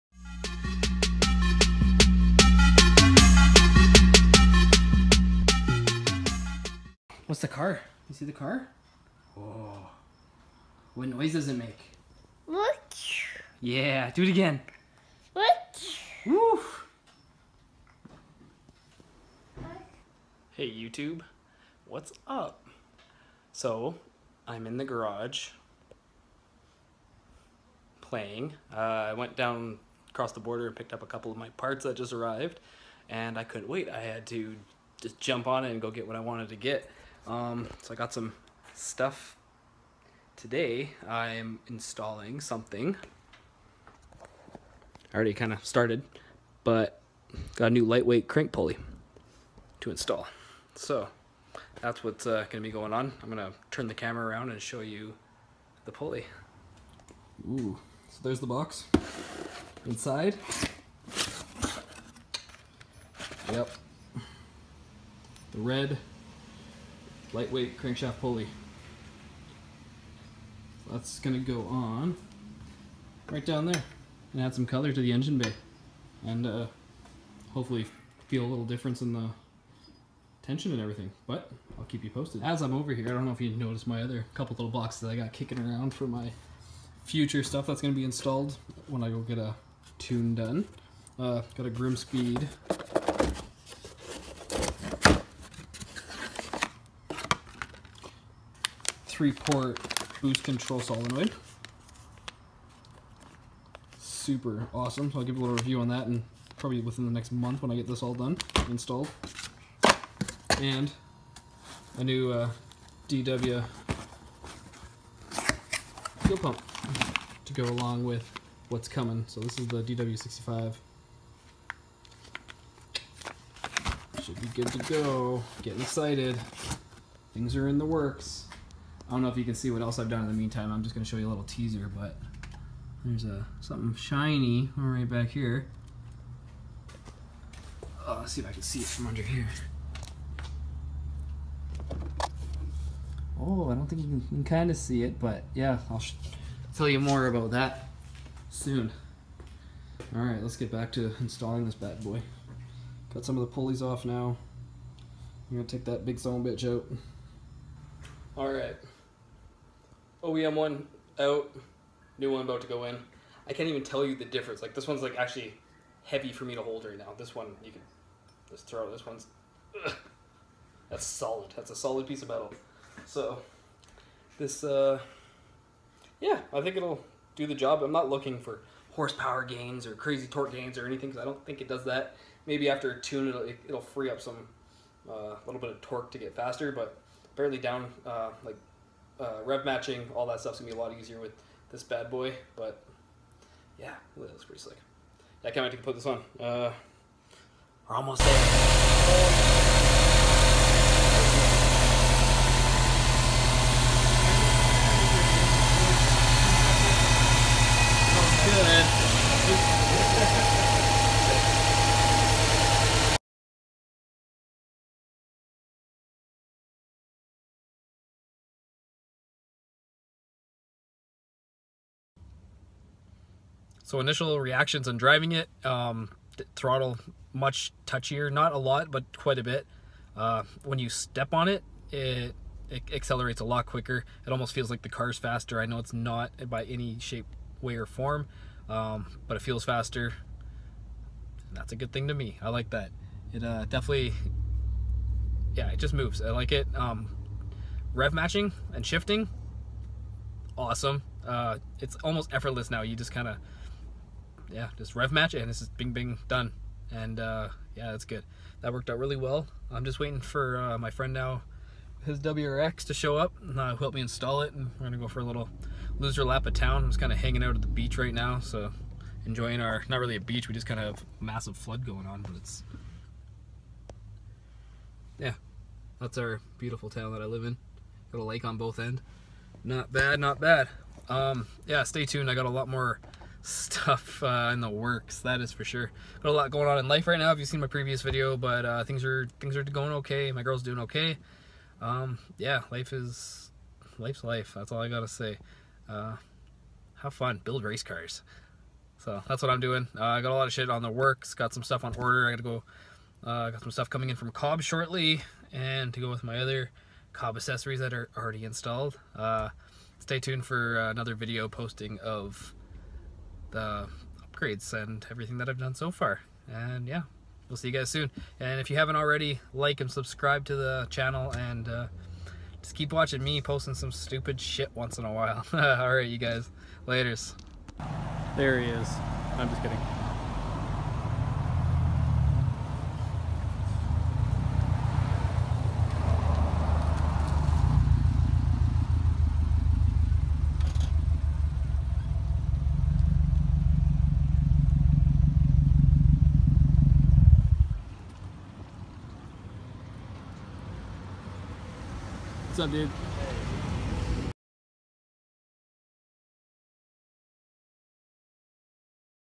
2017 Subaru STi - Perrin Lightweight Crank Pulley
Just purchased a Perrin Lightweight Crank Pulley. Just giving it a little show of it and how I think it is performing.